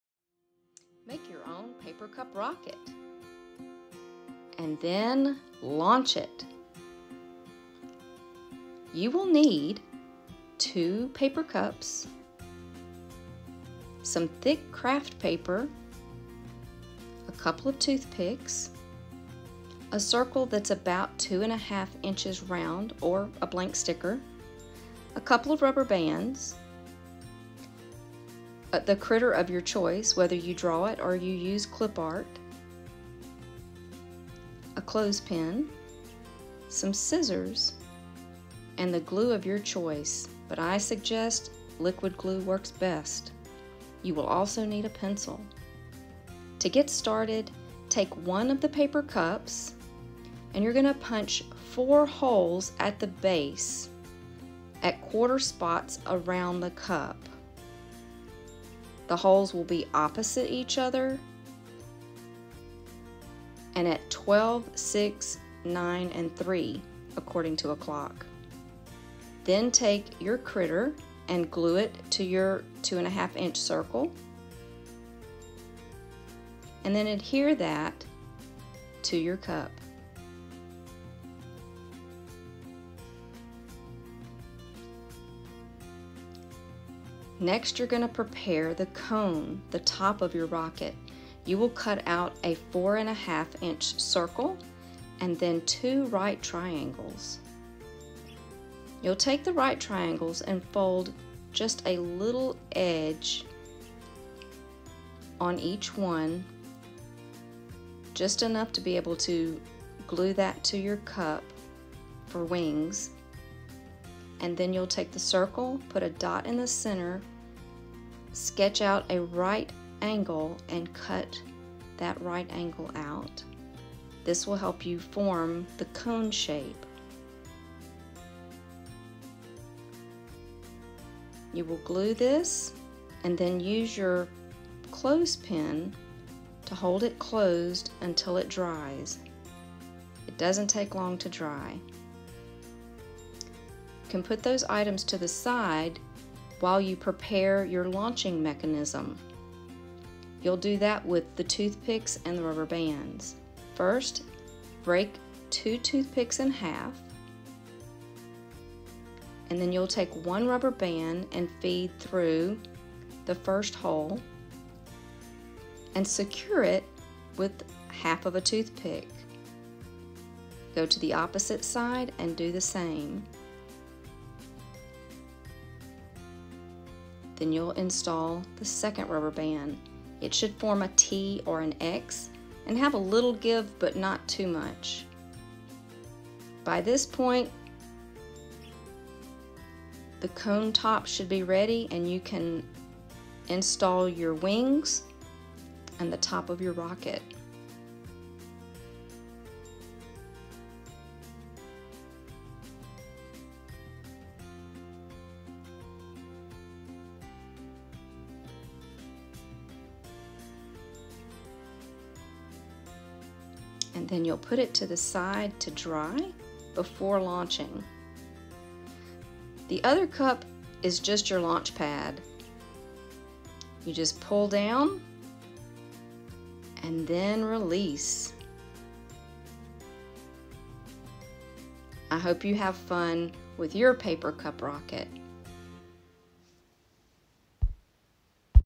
EASY DIY Cup Rockets for kids
Looking for a fun STEM activity? Join Ms. Angie in making paper cup rockets, and launch your favorite critter into space!

If you decide you want to try to build a variety of rocket designs, check out these different types on sciencebuddies.org.